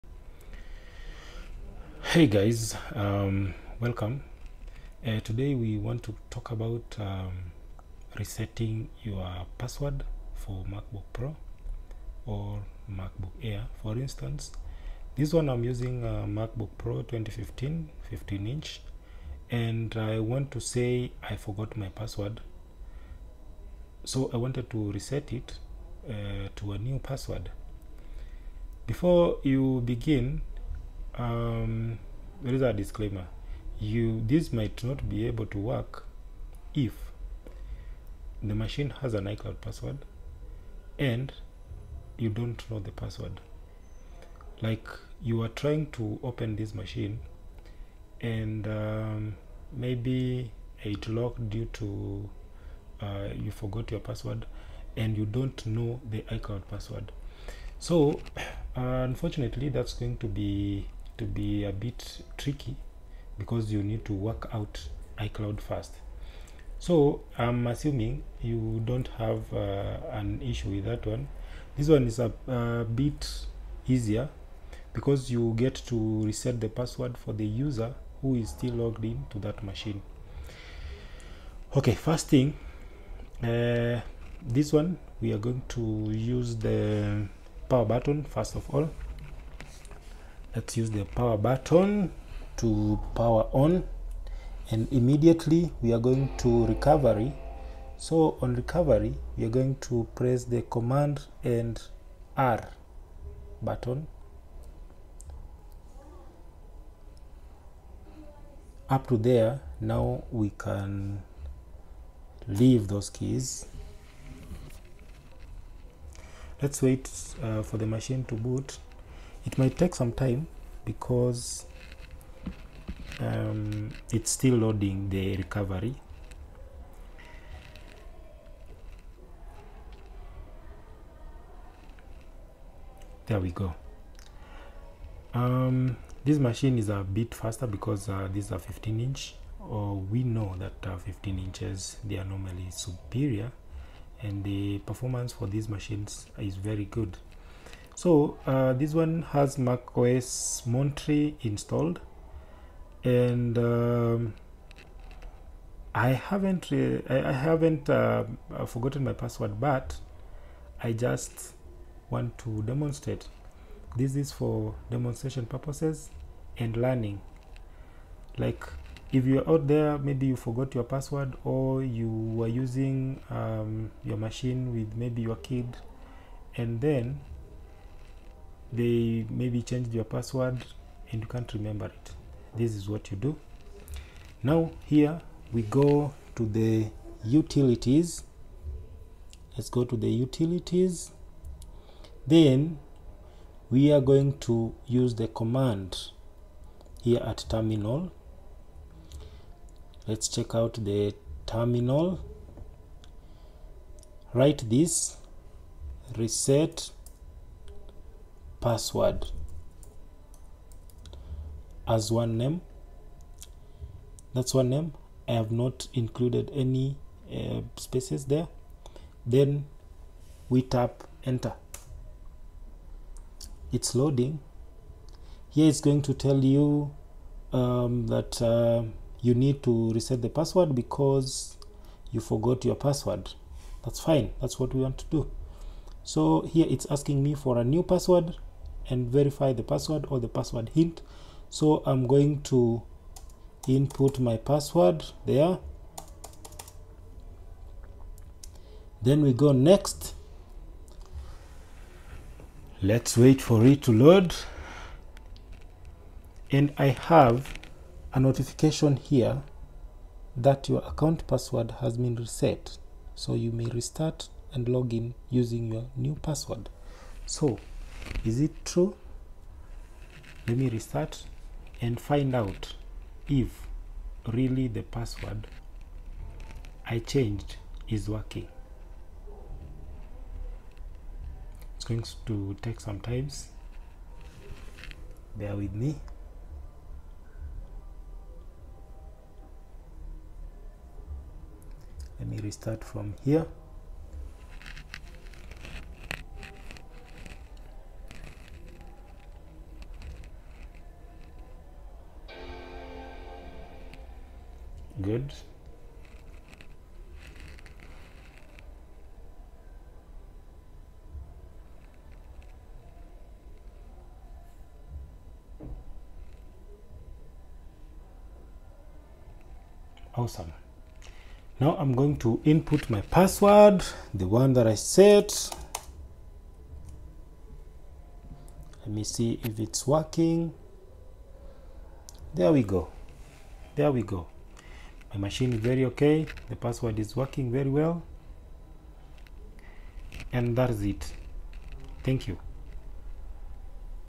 Forgot Password? Reset Your MacBook Pro or MacBook Air Password in terminal - Easy
This is a short video of how to reset your MacBook Pro or MacBook Air passwords under macOS recovery.

￼ This is the easiest process ever and it works hundred percent.